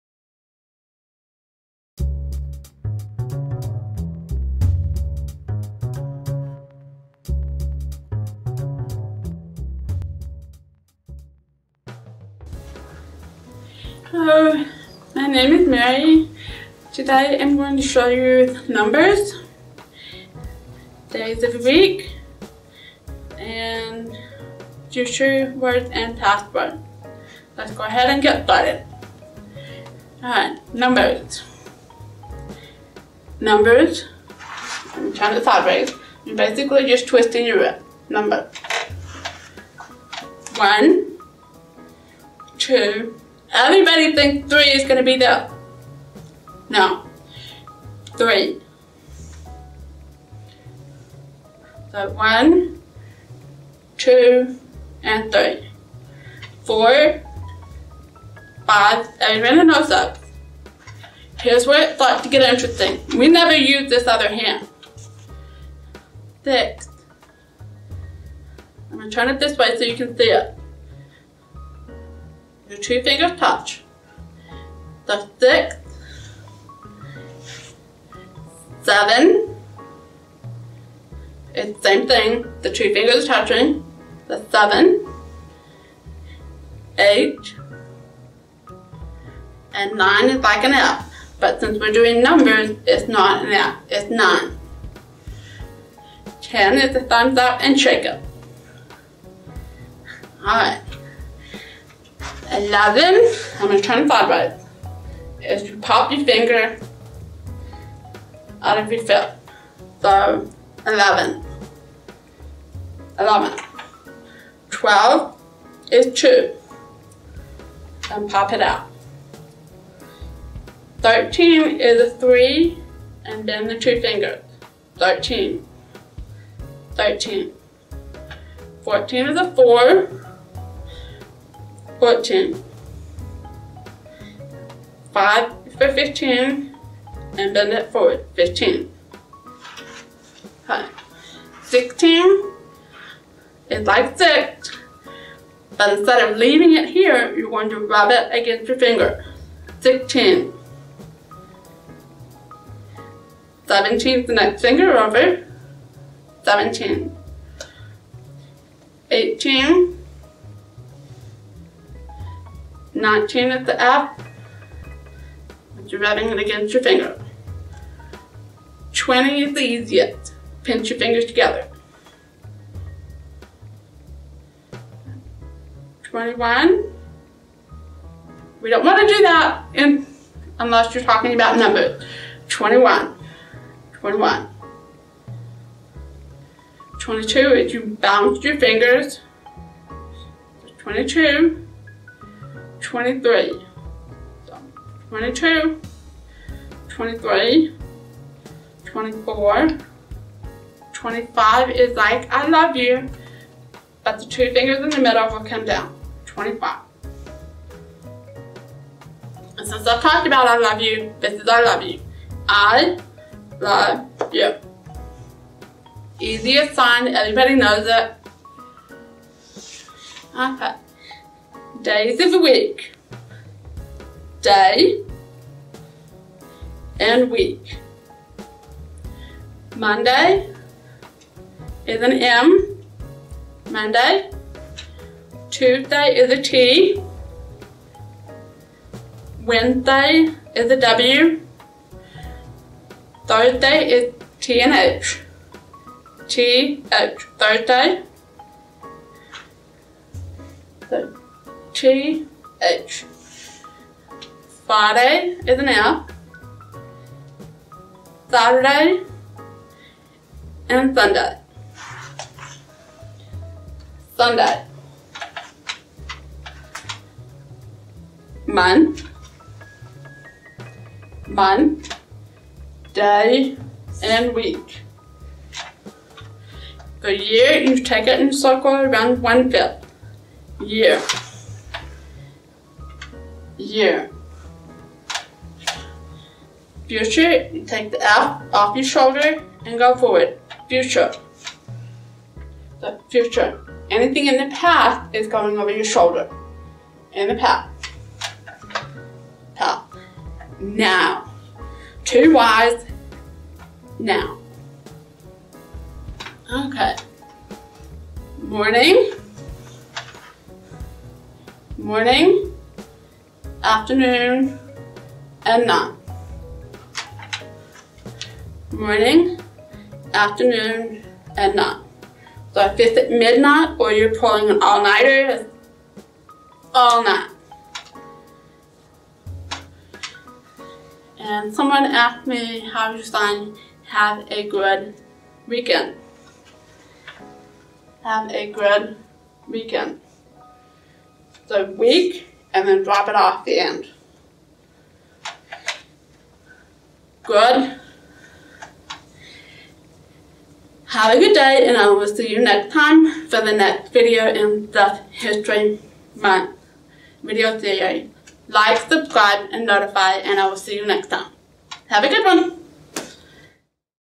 ASL Numbers and More!
Learn to count in American Sign Language.